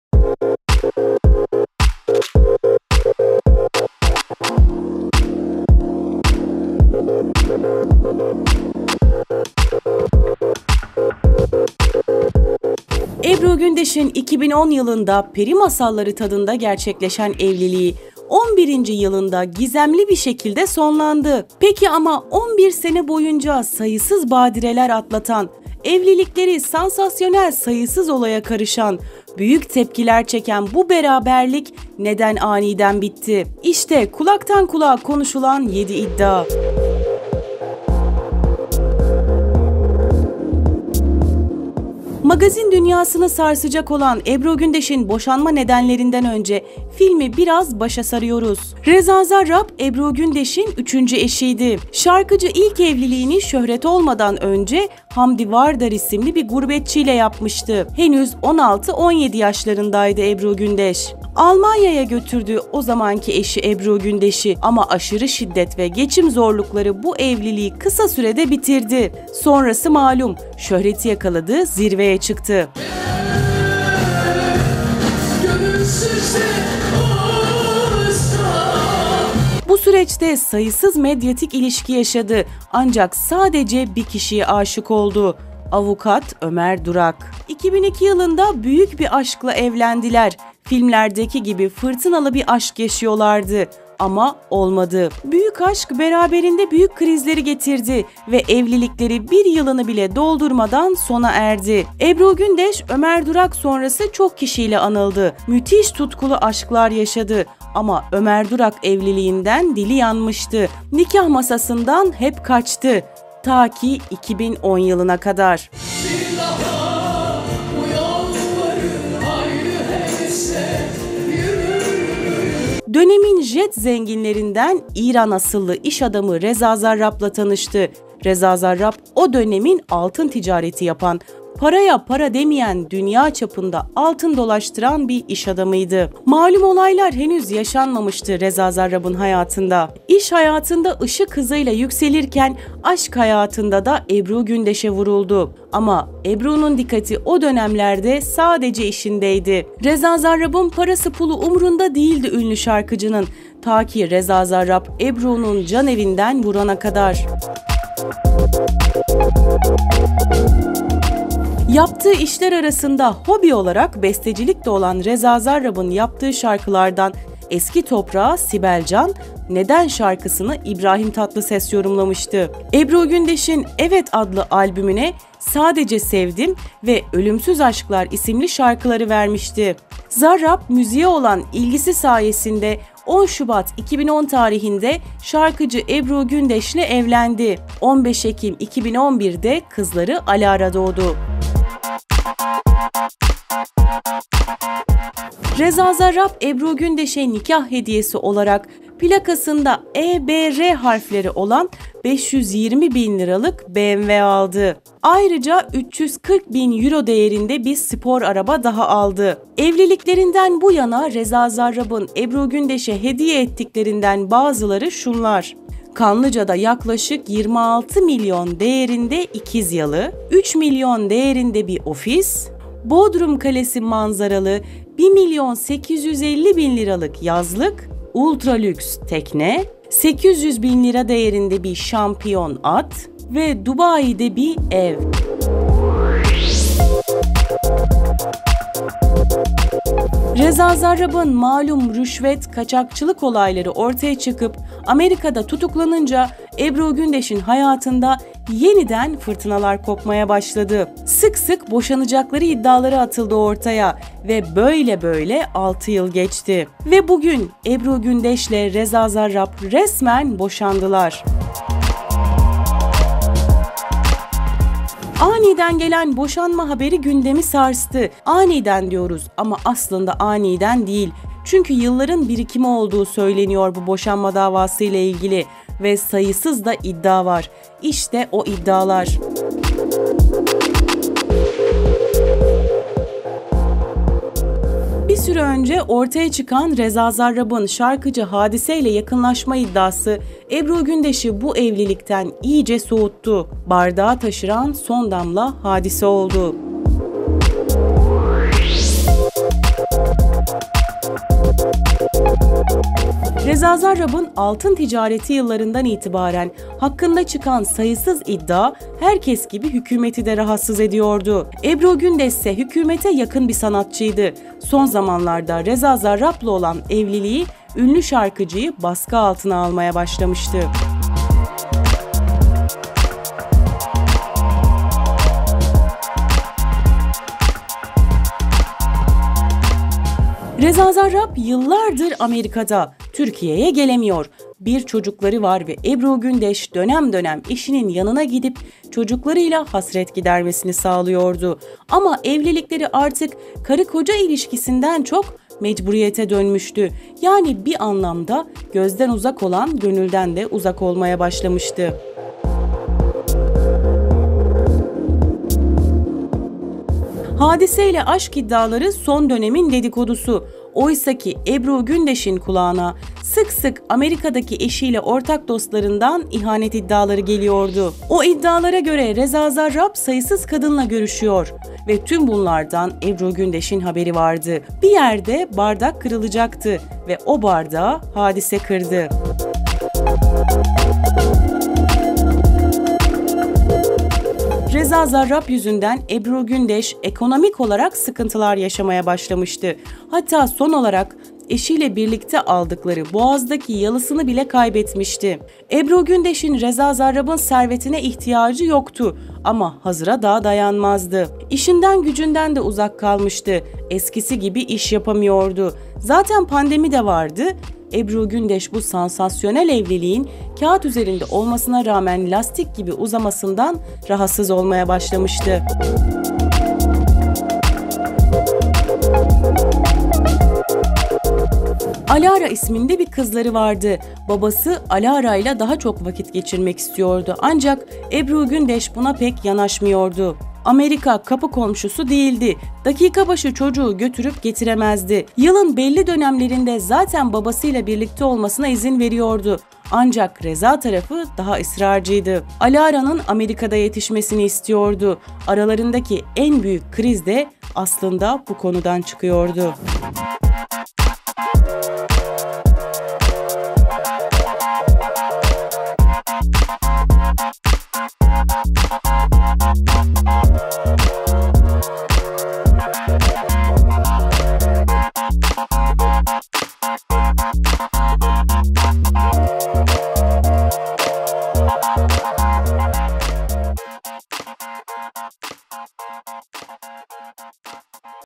Ebru Gündeş ve Reza Zarrab Neden Boşandı?| İşte Tüm Gerçekler...
EBRU GÜNDEŞ NEDEN BOŞANDI? İŞTE ÇOK KONUŞULACAK 7 İDDİA!
Ebru Gündeş’in 2010 yılında “Peri masalları” tadında gerçekleşen evliliği 11. yılında gizemli bir şekilde sonlandı. Peki ama 11 sene boyunca sayısız badireler atlatan, evlilikleri sansasyonel sayısız olaya karışan, büyük tepkiler çeken bu beraberlik neden aniden bitti. İşte kulaktan kulağa konuşulan 7 iddia.